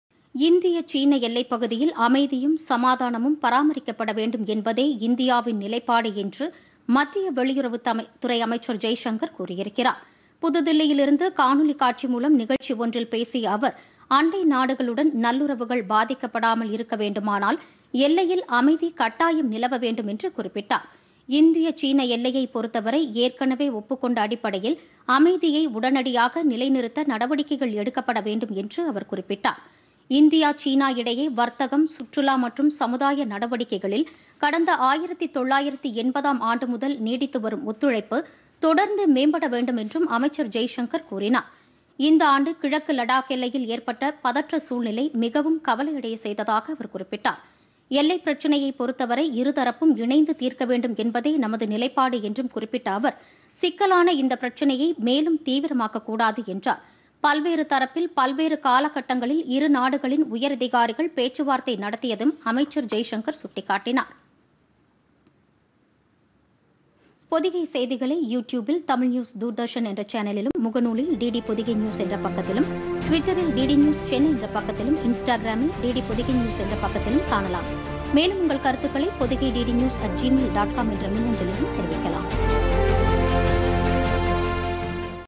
எல்லையில் அமைதி நிலவ வேண்டும்PodhigaiTamilNews
இந்திய - சீன எல்லையில் அமைதி நிலவ வேண்டும் என்பதே இந்தியாவின் நிலைப்பாடு - வெளியுறவுத்துறை அமைச்சர் ஜெய்சங்கர்.